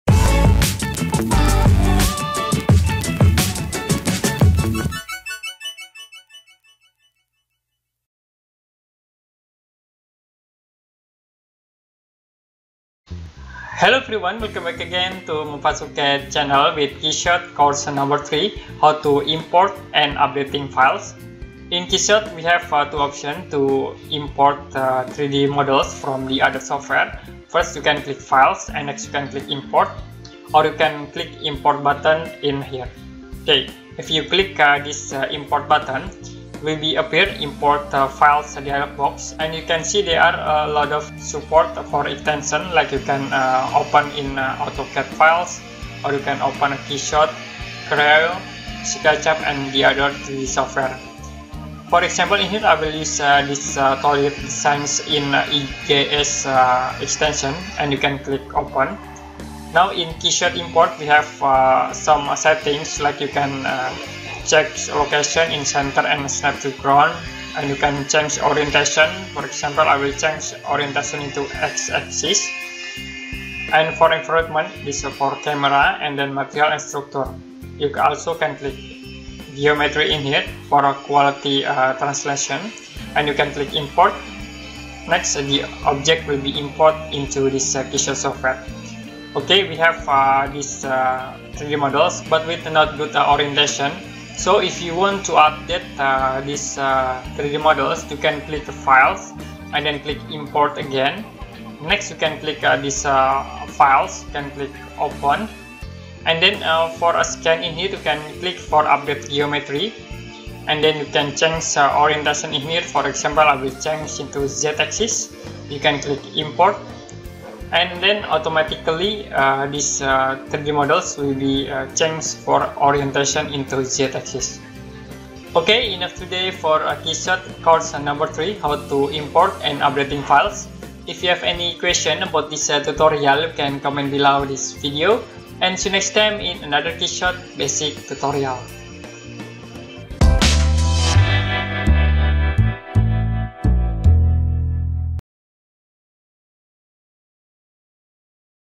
#3 Import And Updating Model Keyshot Course For Beginner
Learn keyshot course 3 how to import and updating model with some basic settings, check it out!!!